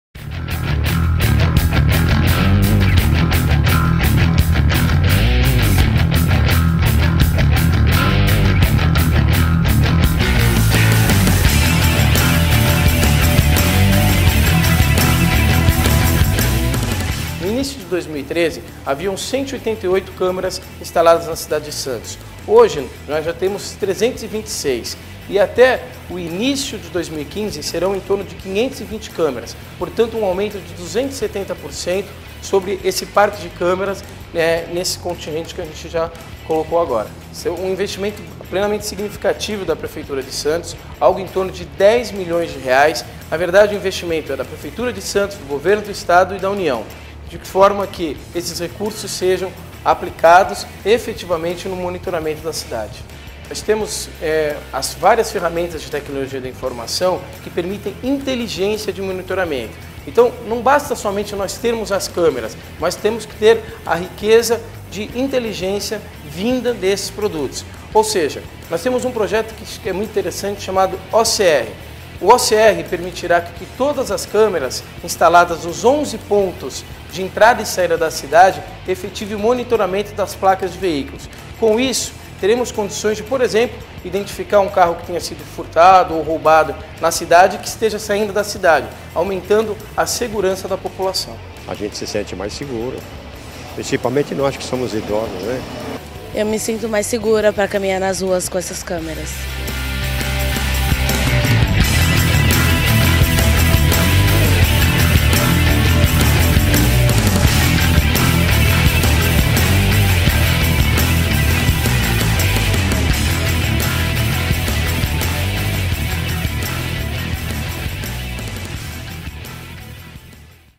Cidade monitorada: Santos investe em câmeras para mais segurança
Até 2015 serão 520 câmeras instaladas em pontos estratégicos da Cidade, totalizando cerca de R$ 10,5 milhões de investimentos em segurança pública.  O SIM - Sistema Informatizado de Monitoramento, fica no paço municipal, funciona 24h e tem a atuação de guardas municipais, guardiões cidadãos, policiais militares e agentes da CET.  Denúncias podem ser feitas pelo 153 e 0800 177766 (gratuitos).